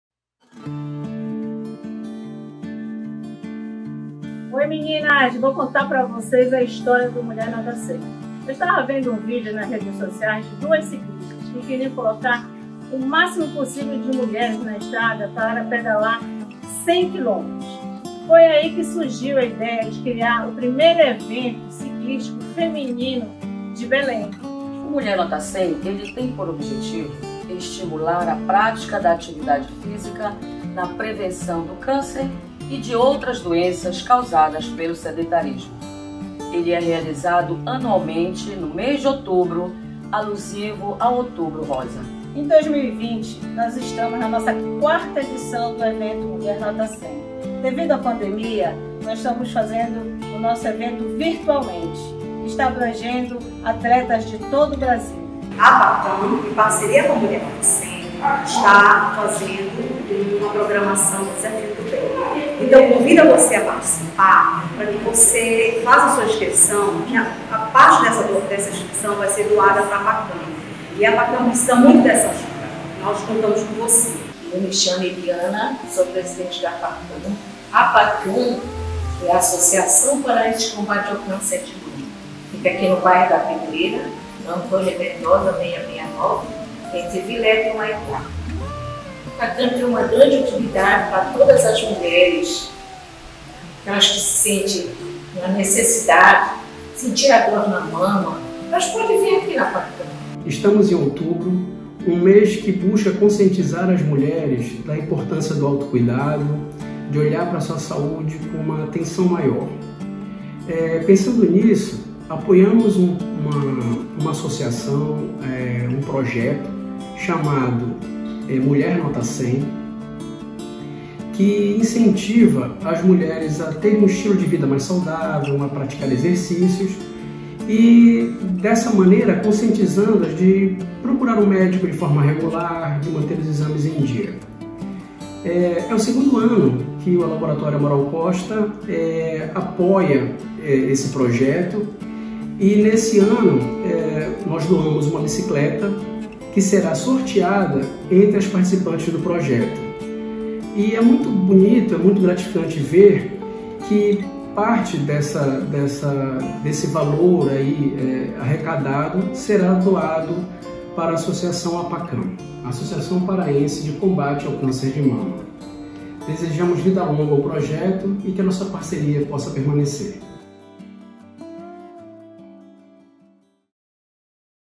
APOIO A MULHERES NA LUTA CONTRA O CÂNCER DE MAMA
Há cerca de um ano, apoiamos pela primeira vez o @mulhernotacem, projeto que incentiva a vida saudável e a prática de esportes, como o ciclismo. Neste OutubroRosa, convidamos algumas integrantes para compartilhar suas histórias e mostrar como incentivam outras. Assista e conheça o evento, que este ano doará parte das inscrições para a Associação Paraense de Combate do Câncer de Mama (APACAM).